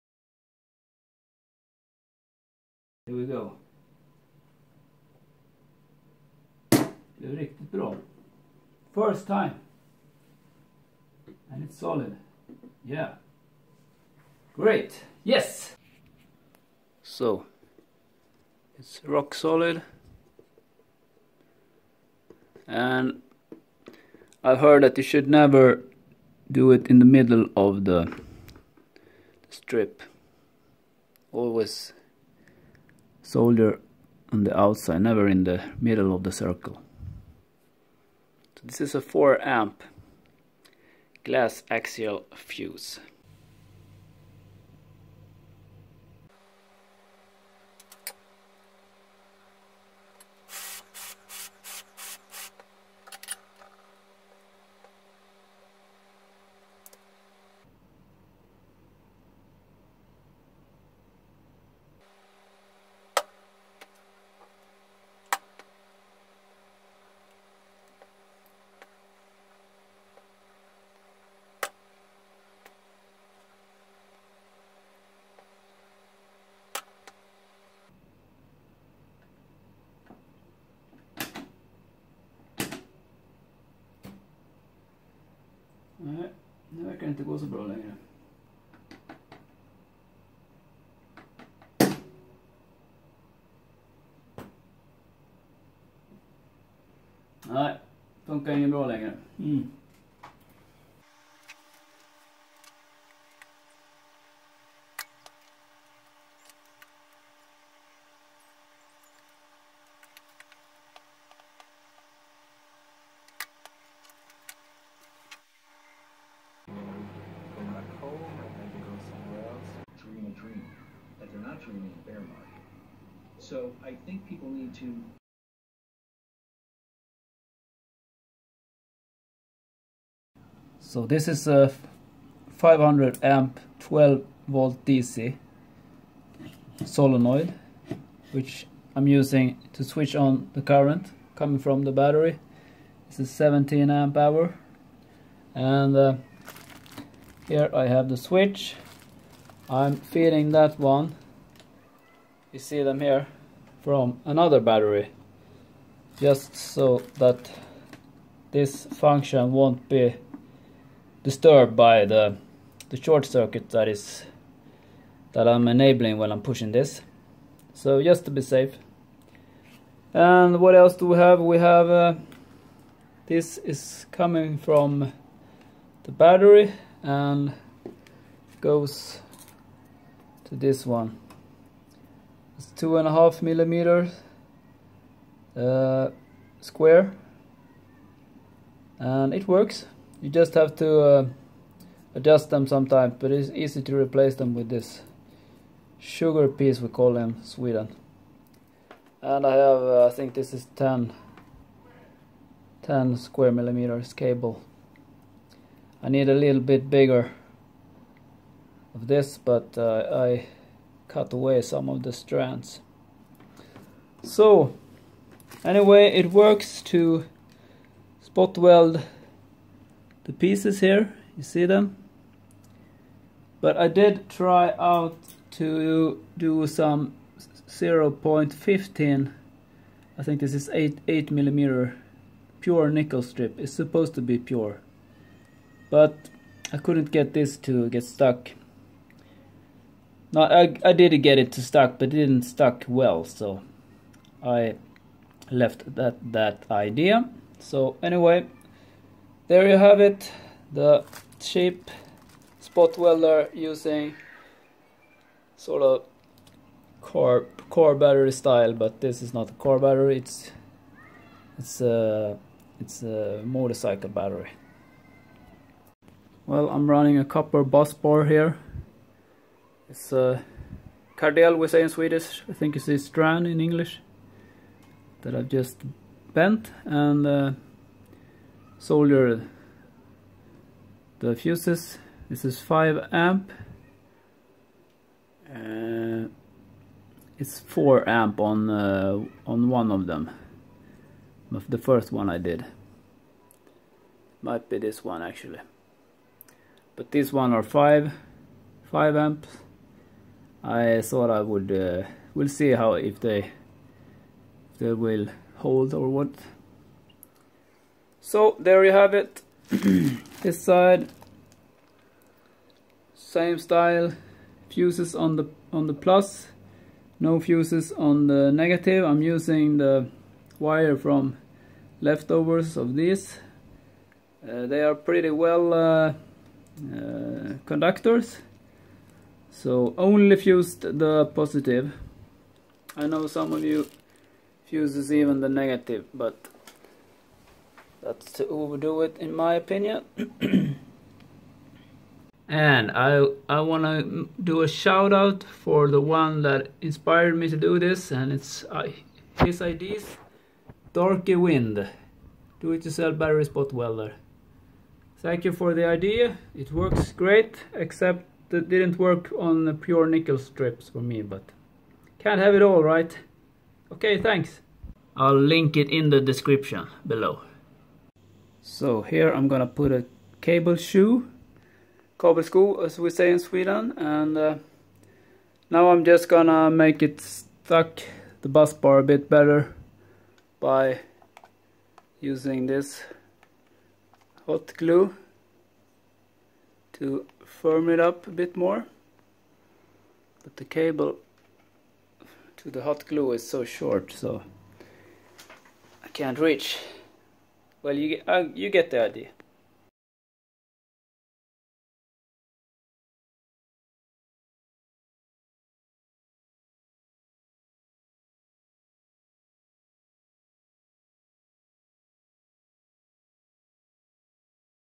Making a 3S12P DIY 12.6V Li-Ion BATTERY using 18650
Using 5amps fuses for each cell. This is 3S12P battery.

Bussbar, spot welding and soldering